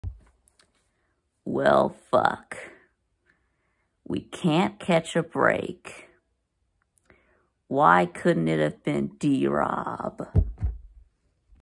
George Pickens Tears ACL in Practice
Why couldn’t it have been D Rob!?!?!😭😭😭